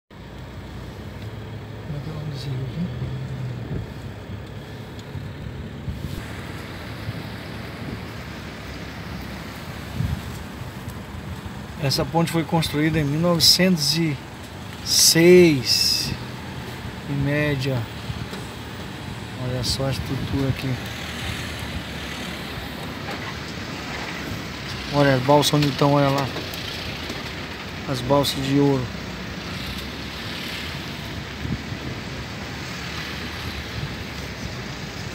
PONTE DA ANTIGA ESTRADA DE FERRO - GUAJARÁ MIRIM
Ponte na BR 425, que liga Porto Velho e Gujara Mirim, fronteira com a Bolívia, Ponte da antiga Estrada de Ferro Madeira Momoré, foi construída entre os anos de 1905 a 1912, período da construção da Estrada de Ferro, era usada pelo trem que fazia a condução da borracha de Guajará Mirim até o porto da Cidade de Porto Velho, hoje esta ponte é utilizada na BR 425 que liga a Capital, Porto Velho a Cidade de Guajará Mirim que faz fronteira com a Bolívia.

Compartilhe
Deixe o seu comentário
VAMOS VIAJAR PELO BRASIL E O MUNDO
O BRASIL É NOSSO
Marcos & Cleide